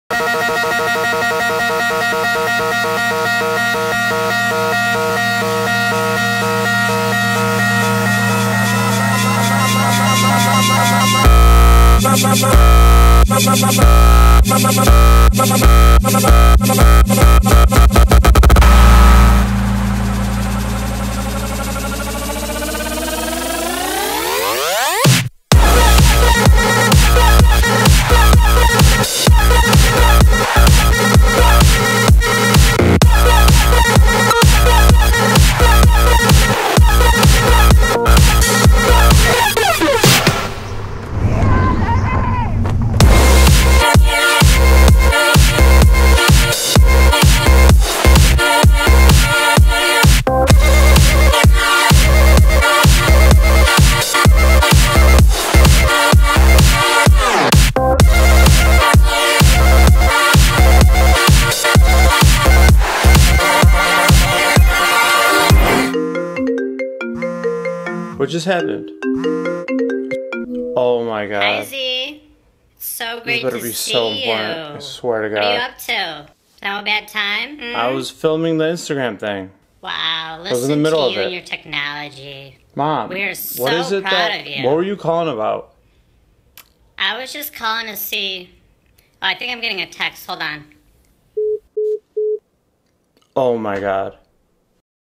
How To Experience: INSTAGRAM Face Filters
Every instagram filter in (almost) 1 minute.